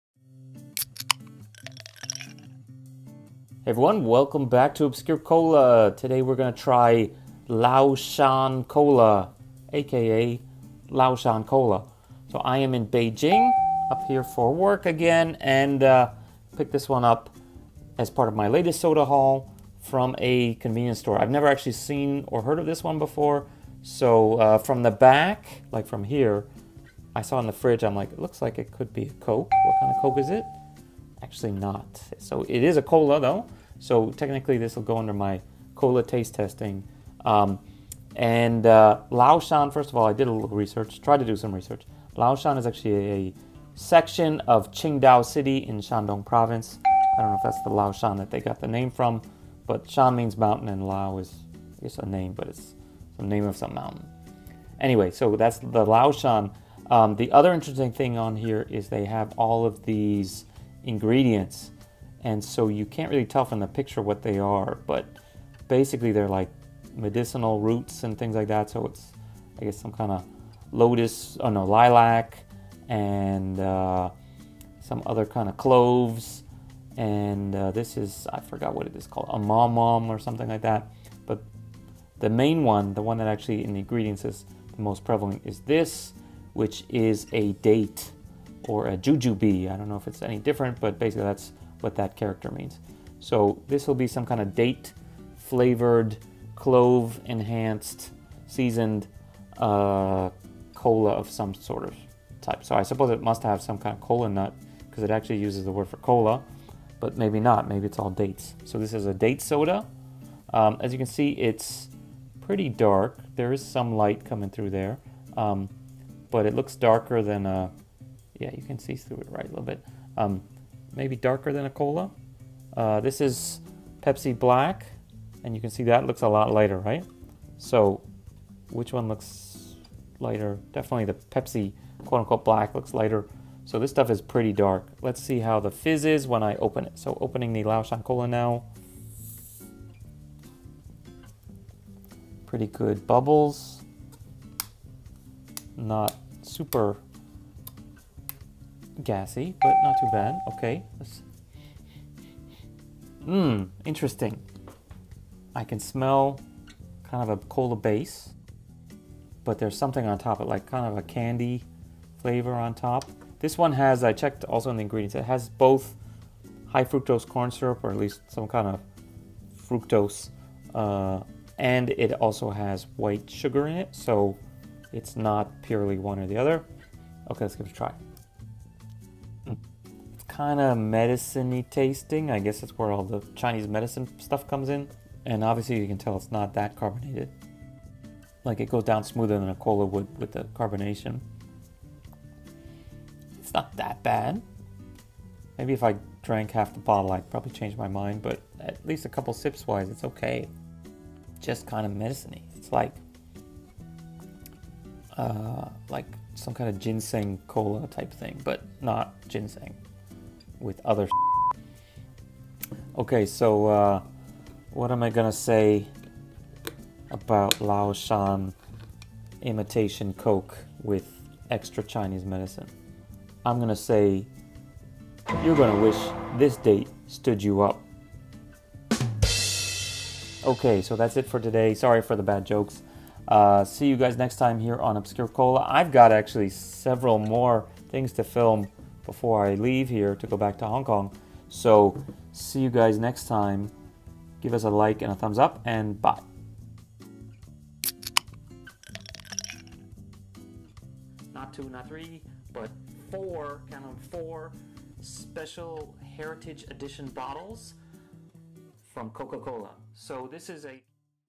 What Does Lao Shan Cola Taste Like? China Cola Taste Test | Obscure Cola
it looks like Coke, but it ain't coke!  It's Laoshan Cola (崂山可乐) from China.  Made with dates!